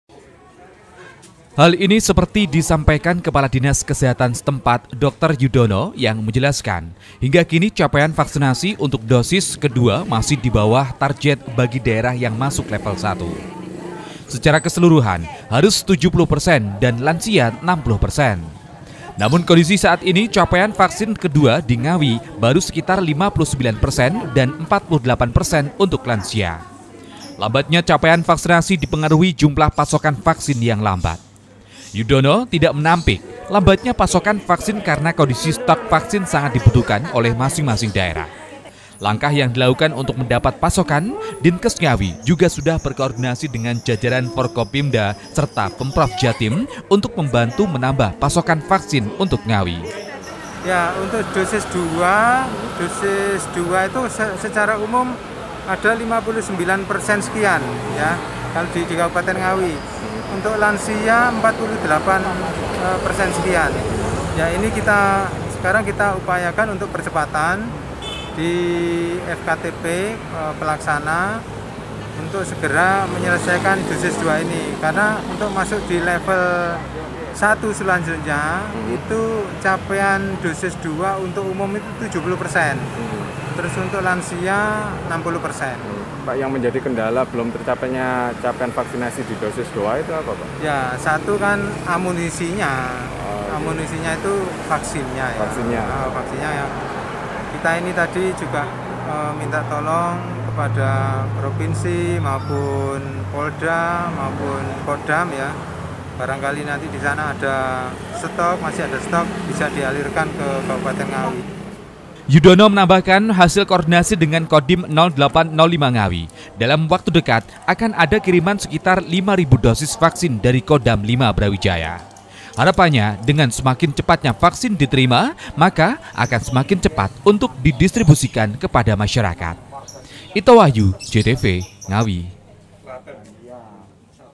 NGAWI - DINKES Mulai Kesulitan Dapat Pasokan Vaksin
Capaian vaksinasi di kabupaten ngawi belum sampai 70 persen. Untuk vaksinasi dosis kedua baru mencapai total 59 persen dengan total vaksinasi lansia dosis kedua 48 persen.  Dinkes setempat menilai terlambatnya pasokan vaksin ini menjadi penyebab belum tercapainya target vaksinasi.